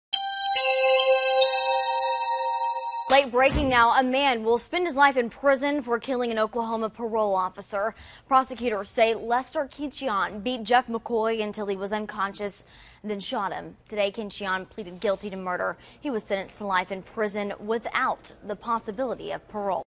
Kinchion pleads guilty to killing parole officer
Lester Kinchion pleaded guilty Thursday and was sentenced to life for killing Jeff McCoy, an Oklahoma Department of Corrections parole officer.